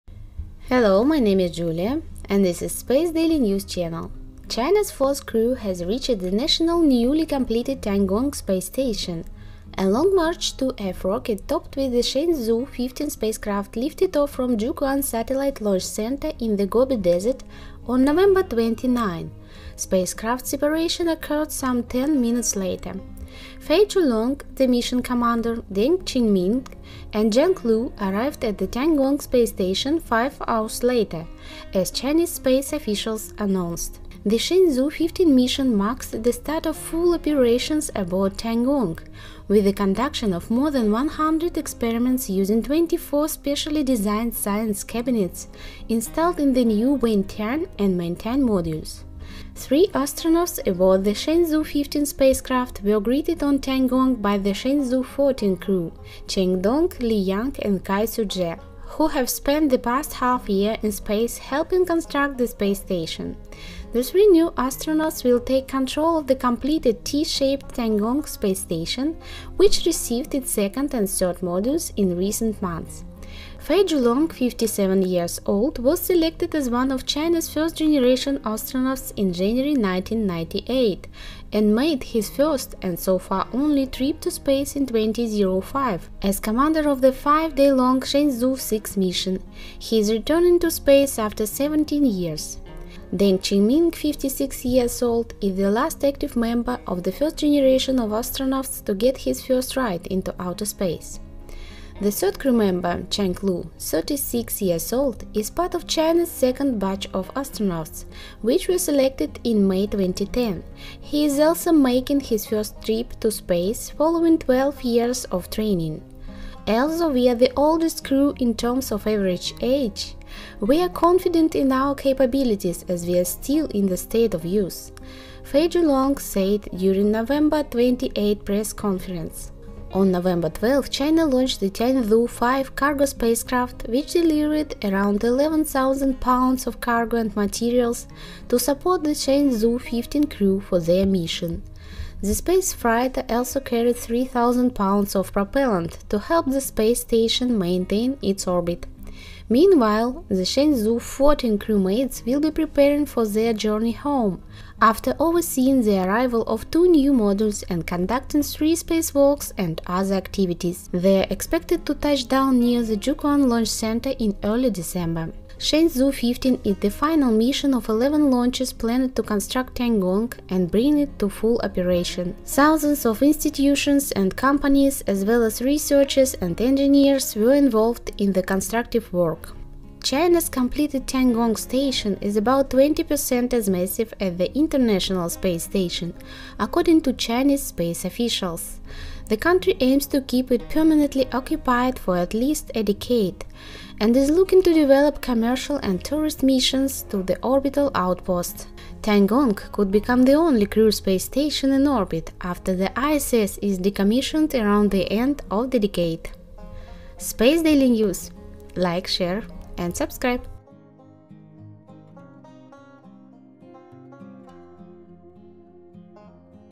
Shenzhou 15 astronauts arrive at China’s space station for first crew handover [space news]
China's fourth crew has reached the nation's newly completed Tiangong space station.

A Long March 2F rocket topped with the Shenzhou 15 spacecraft lifted off from Jiuquan Satellite Launch Center in the Gobi Desert on Nov. 29. Spacecraft separation occurred some 10 minutes later.

Fei Junlong (the mission commander), Deng Qingming and Zhang Lu arrived at Tiangong space station 5 hours later, as Chinese space officials announced.

The Shenzhou 15 mission marks the start of full operations aboard Tiangong, with the conduction of more than 100 experiments using 24 specially designed science cabinets installed in the new Wentian and Mengtian modules.

Three astronauts aboard the Shenzhou 15  spacecraft  were greeted on Tiangong by the Shenzhou 14 crew — Chen Dong, Liu Yang and Cai Xuzhe — who have spent the past half year in space helping construct the space station. The three new astronauts will take control of the completed, T-shaped Tiangong space station, which received its second and third modules in recent months.

Fei Junlong, 57, was selected as one of China's first-generation astronauts in January 1998 and made his first, and so far only, trip to space in 2005 as commander of the five-day-long Shenzhou 6 mission. He is returning to space after 17 years.

Deng Qingming, 56, is the last active member of the first generation of astronauts to get his first ride into outer space. The third crew member, Zhang Lu, 46, is part of China’s second batch of astronauts, which were selected in May 2010. He is also making his first trip to space, following 12 years of training.

"Although we are the oldest crew in terms of average age, we're confident in our capabilities, as we are still in the 'state of youth,'" Fei Junlong said during a Nov. 28 press conference.

On Nov. 12, China launched the Tianzhou 5 cargo spacecraft, which delivered around 11,000 pounds (5,000 kilograms) of cargo and materials to support the Shenzhou 15 crew for their mission.

The space freighter also carried 3,080 pounds (1,400 kg) of propellant to help the space station maintain its orbit.

Meanwhile, the Shenzhou 14 crewmates will be preparing for their journey home, after overseeing the arrival of two new modules and conducting three spacewalks and other activities. They are expected to touch down near the Jiuquan launch center in early December.

Shenzhou 15 is the final mission of 11 launches planned to construct Tiangong and bring it to full operation.

Thousands of institutions and companies, as well as researchers and engineers, were involved in the constructive work.

China's completed Tiangong station is about 20% as massive as the International Space Station (ISS), according to Chinese space officials. The country aims to keep it permanently occupied for at least a decade and is looking to develop commercial and tourist missions to the orbital outpost.

Tiangong could become the only crewed space station in orbit after the ISS is decommissioned around the end of the decade.